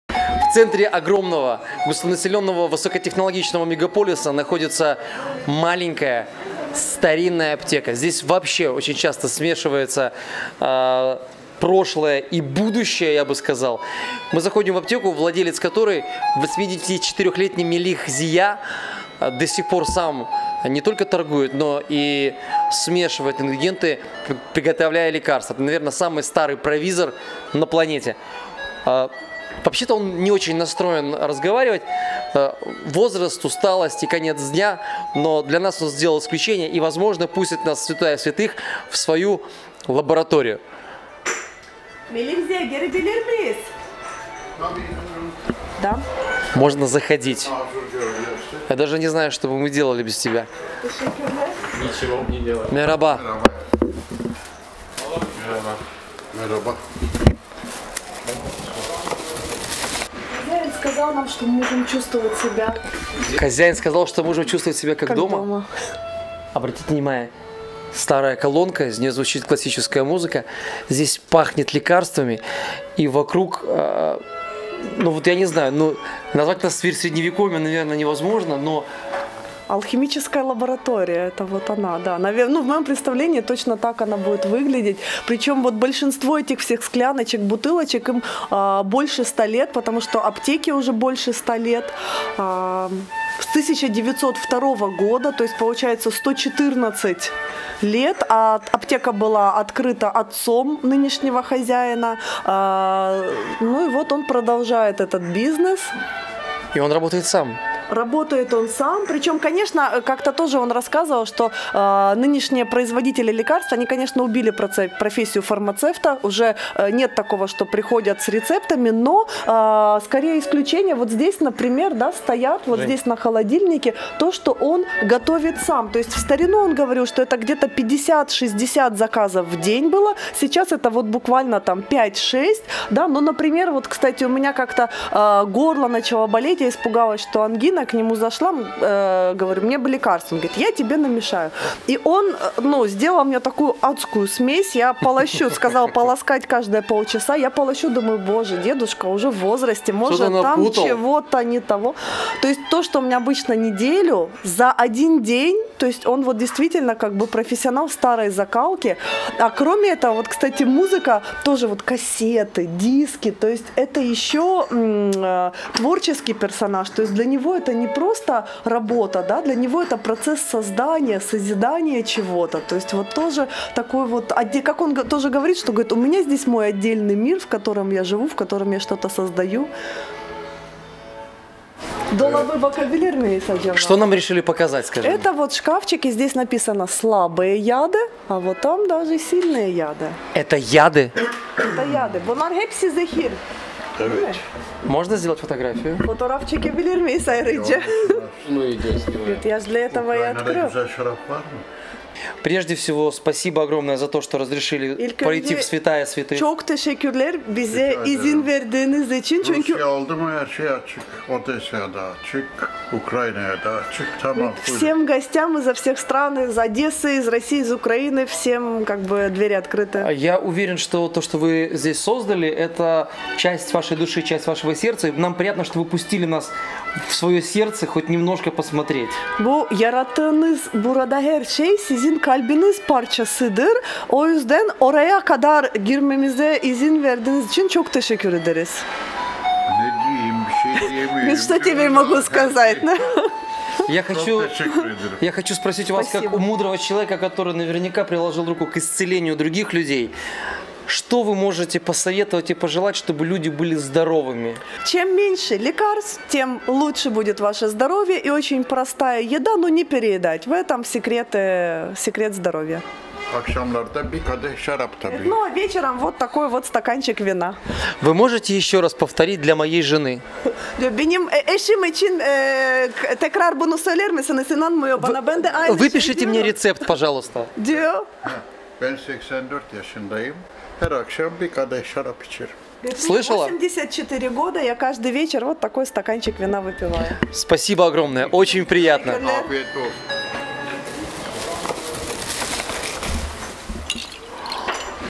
История самой старой аптеки Стамбула
GreenJek channel гордится такими репортажами. Мы не только делимся уникальными историями, но и показываем тех людей, которых вы уже вряд ли встретите в свое жизни. В этот раз мы попали в старинную аптеку Стамбула и познакомились самым старым провизором. Это не просто история, это память о таких замечательных людях профессии, которых исчезают с нашей планеты. Огромная благодарность Стамбулке (Василисе Йылмаз), которая поделилась с нами частичкой уникальной истории.

Найдите нас в Facebook и VK!